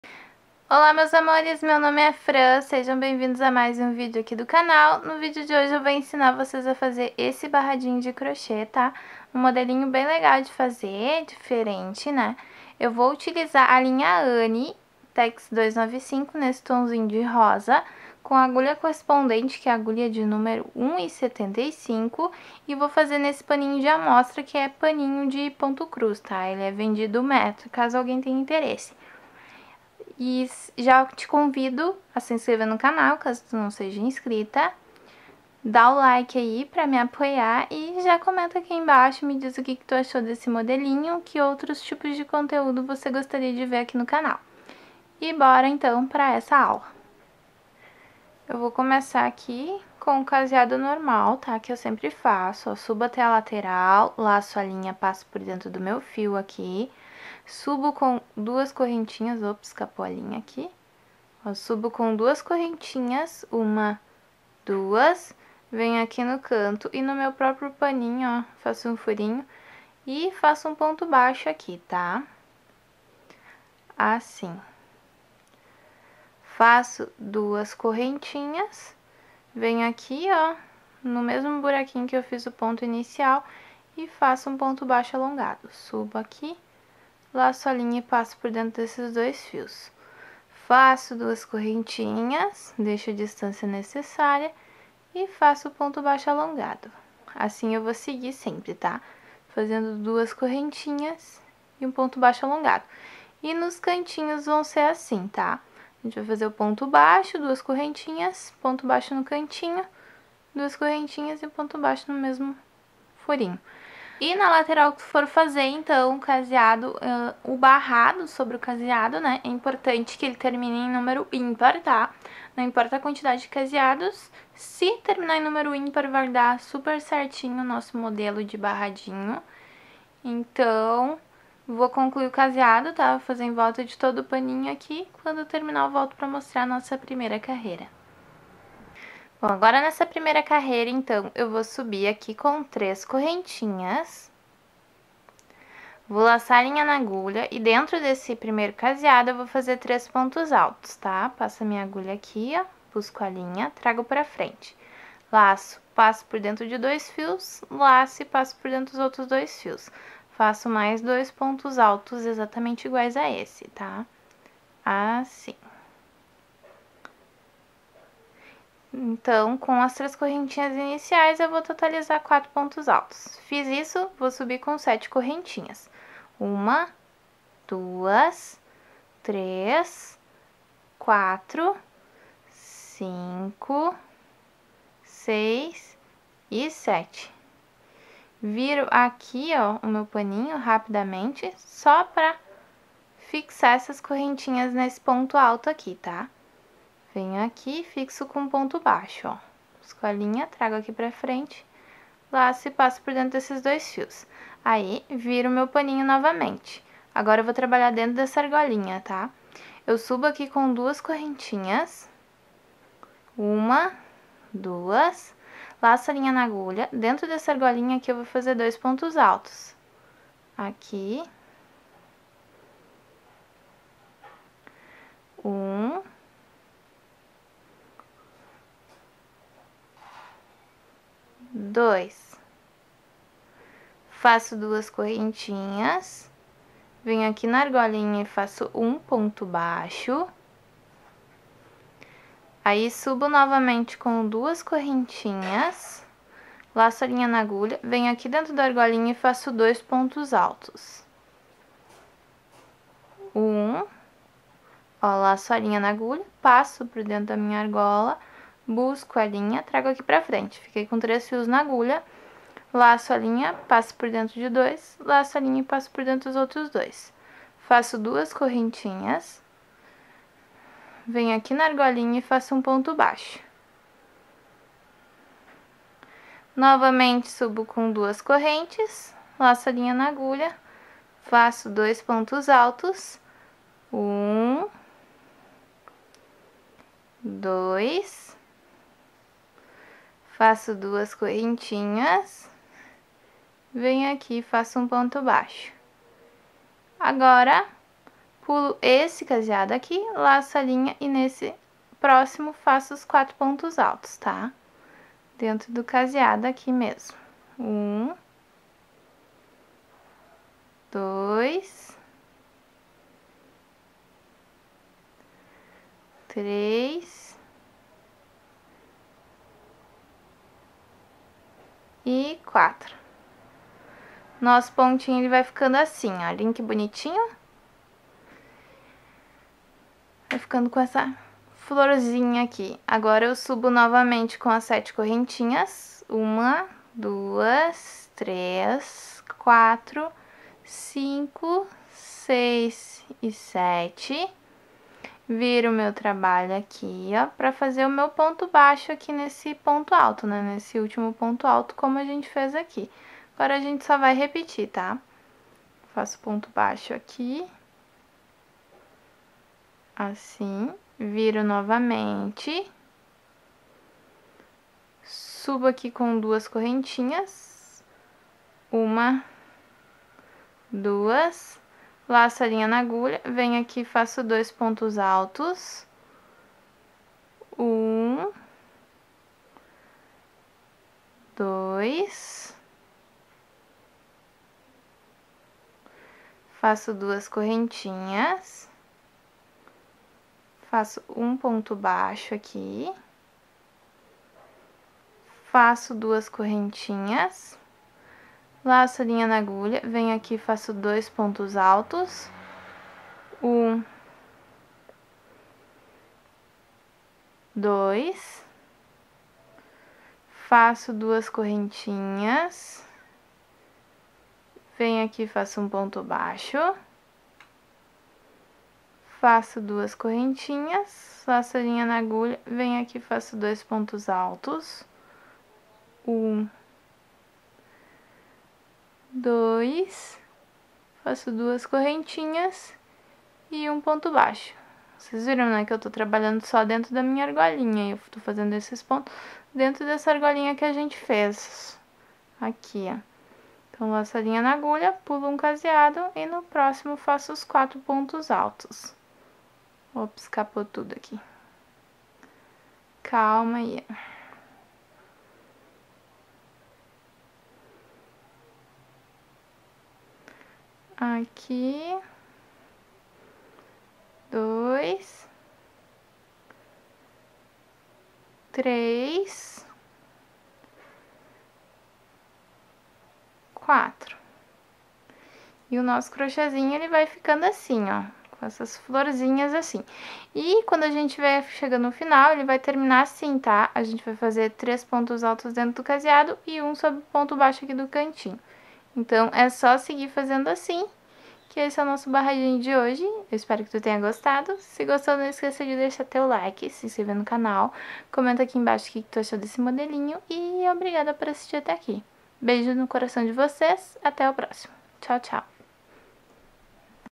BARRADO DE CROCHÊ PARA PANO DE PRATO LINDO E FÁCIL #283 | BICO DE CROCHÊ
No vídeo de hoje compartilho mais um passo a passo de barrado de crochê para pano de prato, toalhas de mesa e qualquer trabalho que você desejar.

Vídeos para iniciantes!

Material utilizado:

- Linha anne tex 295;
- Agulha 1,75 mm;
- Pano de amostra;
- Tesoura.